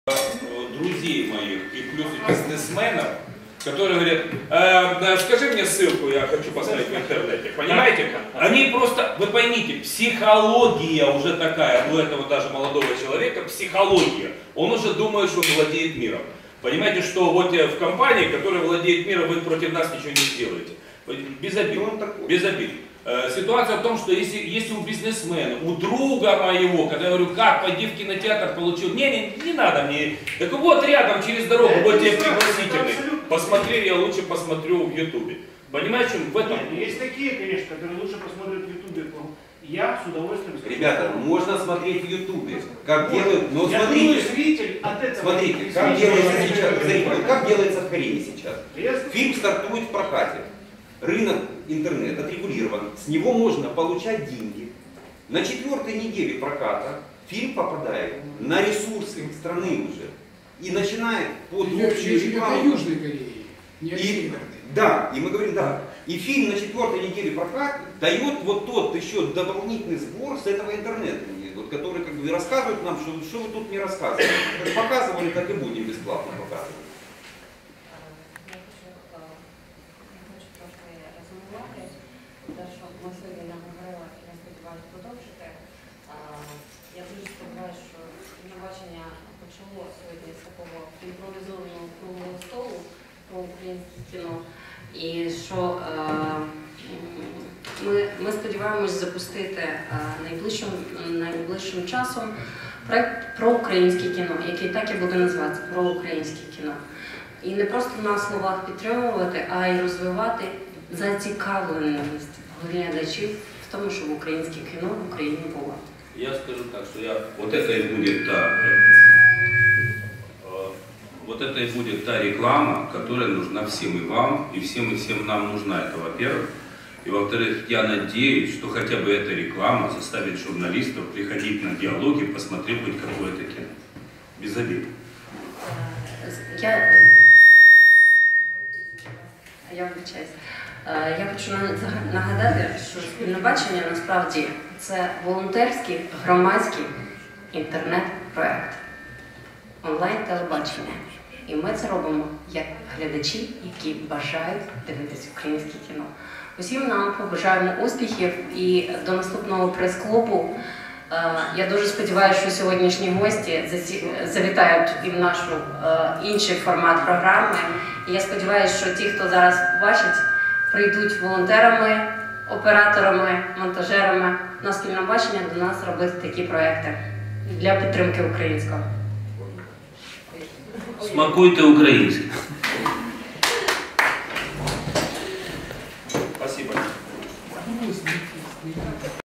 Українське кіно сьогодні. Рух вперед!? - Прес-клуб СПІЛЬНОБАЧЕННЯ та НМПУ (6 частина)
Прес-клуб  на тему  «Українське кіно сьогодні. Рух вперед!?»
Гості прес-клубу:
Андрекс Селіванов - сценарист, продюсер.
Любомир Левицький - режисер, сценарист, продюсер.
Олег Ущенко - письменник, режисер.
Віктор Андрієнко - актор, режисер.
Володимир Філіппов -  кінопродюсер.
Дмитро Колесніков - кінопродюсер.

Модератор дискусії - Юрій Луканов - голова Незалежної медіа профспілки України